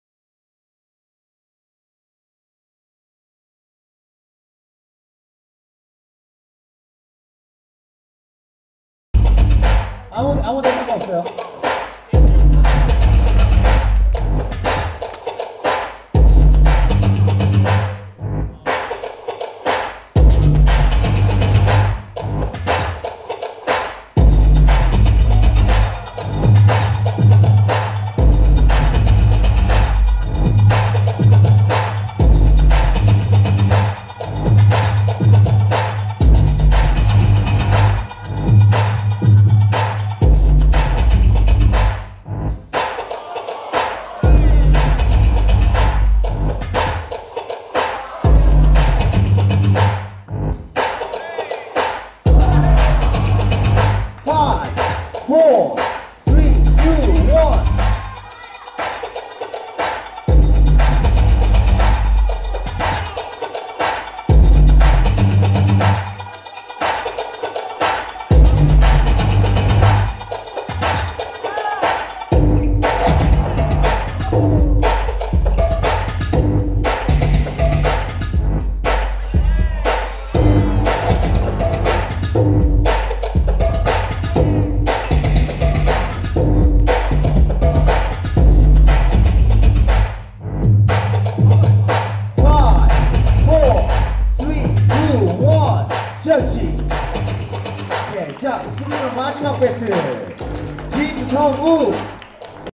SERO vs POPPING CHULO｜1vs1 Best 16 @ KING OF POP vol.2 (2017)｜LB-PIX
King of Pop vol.2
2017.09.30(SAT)
@ Sart College (서울예술실용전문학교 아트홀)

ALL AGES 1 on 1 BATTLE
ROOKIES 2 on 2 BATTLE
ROOKIES 참가 기준 : 청소년 기본법 기준 9세~24세 (24세 포함) 참가 가능

-MC:
 Go
-DJ:
 Mulder
-Judge:
  Rookies 2on2 Battle : Hozin
  All Ages 1on1 Battle : Pete
-Showcase :
 Team Sart
 Funk In The Heart